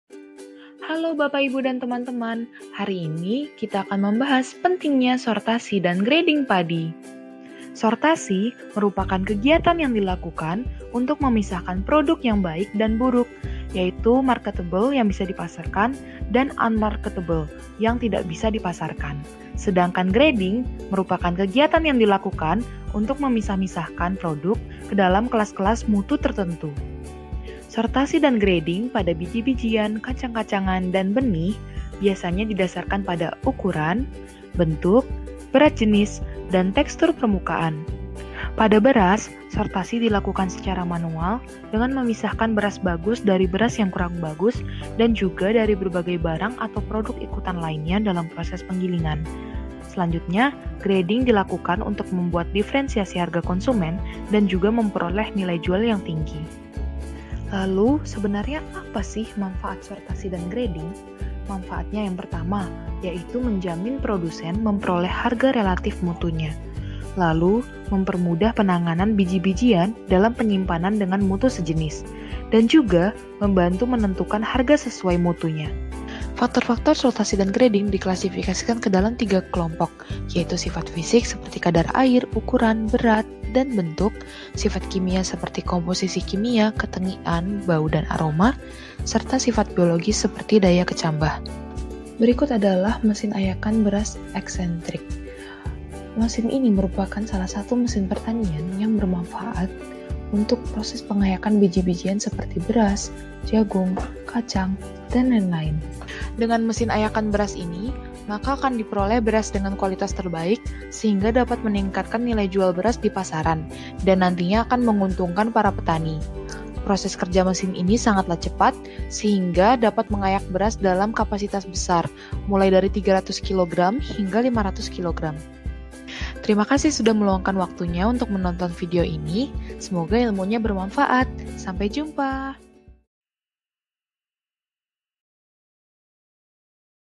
Pentingnya Sortasi dan Grading Padi
Pembinaan teknis pengolahan/penanganan pasca panen sangatlah diperlukan karena sebagian besar petani masih menganggap remeh hal ini, misalnya sortasi dan grading. Apabila sortasi dan grading dilakukan maka para petani dapat menawarkan beberapa mutu kepada konsumen dengan harga yang sesuai dengan mutunya. Menjual dengan variasi harga untuk produk yang sama biasanya dapat memberikan profit yang lebih besar dibandingkan dengan menjual satu jenis komoditas tanpa memilah dan mengelompokkan terlebih dahulu. Selain itu, para petani dapat memperoleh mutu yang lebih baik dan seragam dan memberikan standardisasi dan perbaikan-perbaikan cara pengolahannya dengan melakukan sortasi dan grading.

Rosa Ophelia - Ekonomi Pertanian dan Agribisnis (2017)
KKN-PPM UGM Periode 2 Tahun 2020
Mesuji Timur, Mesuji, Lampung